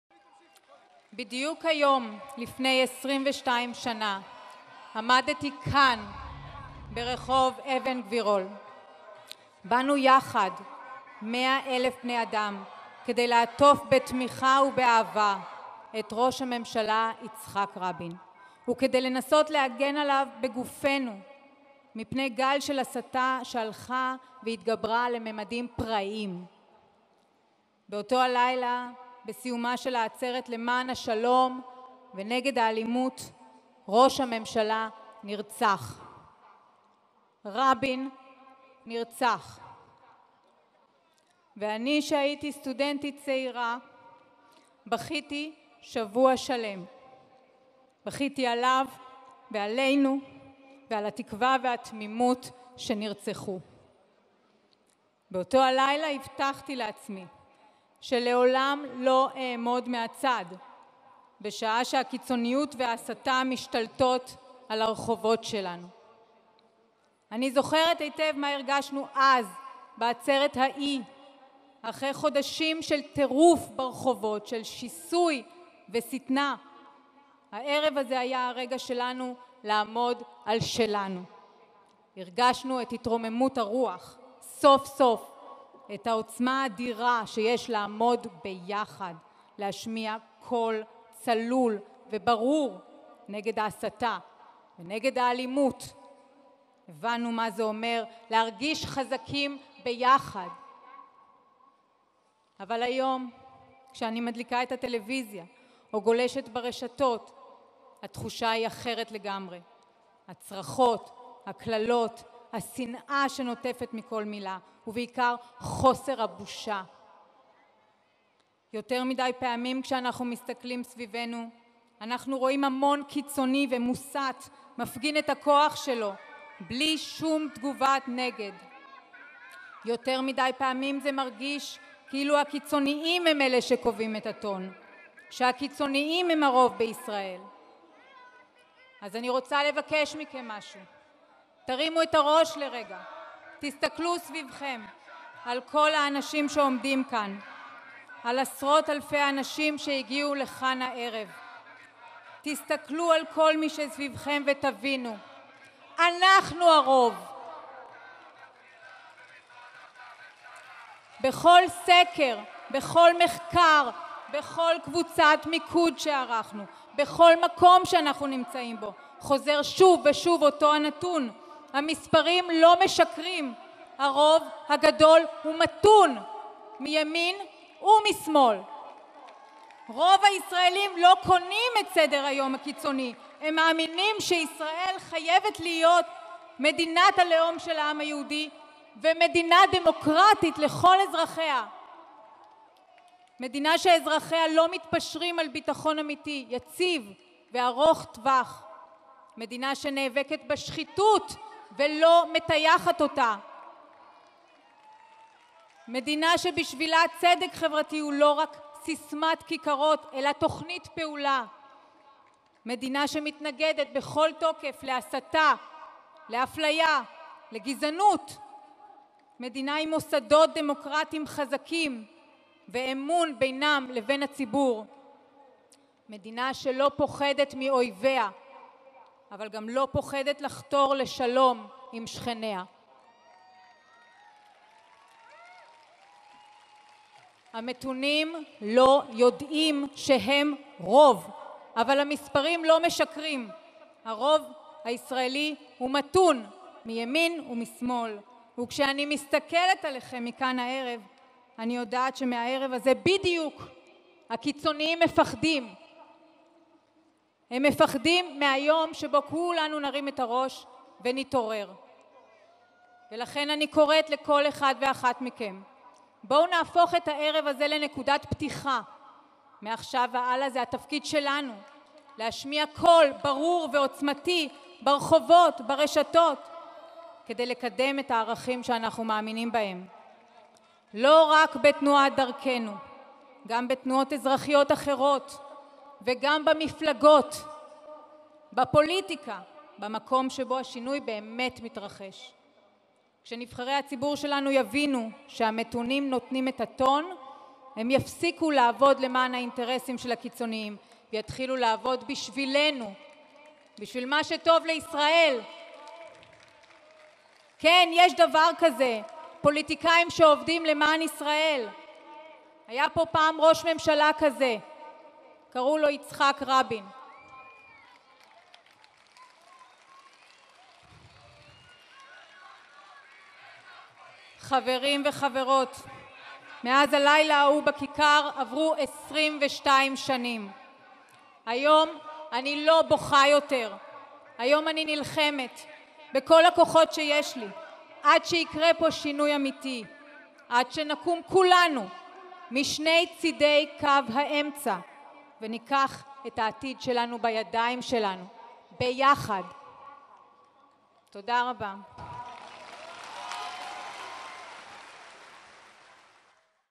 עצרת רבין 2017: פולי ברונשטיין
מנכ״לית תנועת דרכנו בנאומה מתוך עצרת הזיכרון לציון 22 שנים לרצח רה״מ יצחק רבין ז״ל, אשר נערכה ב-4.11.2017 בתל אביב.